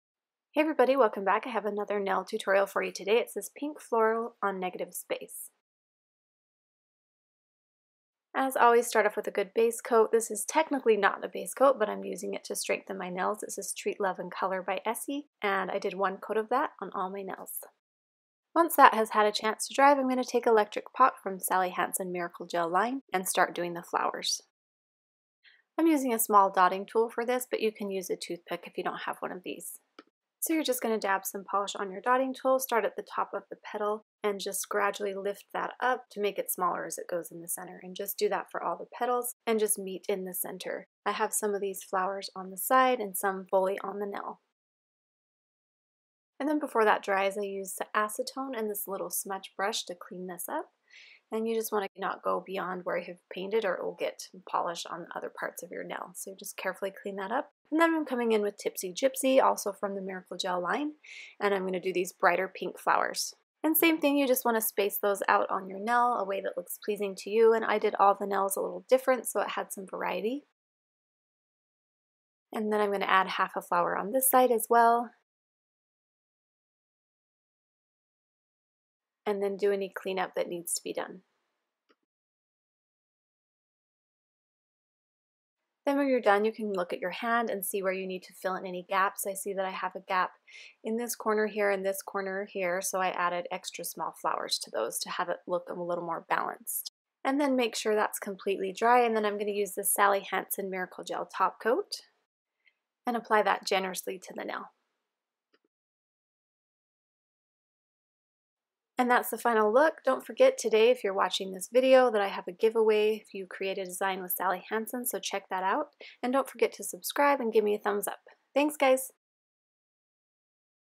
Nail Tutorial: Pink Floral on Negative Space
This spring design is simple to do, try it out for yourself. And I am obsessed with negative space :). If you recreate this design use inspiredbygopolished in Instagram.

PRODUCTS USED:
Sally Hansen Electric Pop:
Sally Hansen Tipsy Gypsy:
Dotting tool:
Essie TLC in Lavendearly:
Sally Hansen Miracle Gel Top Coat:

These products were sent to me for review by Sally Hansen. All opinions are my own.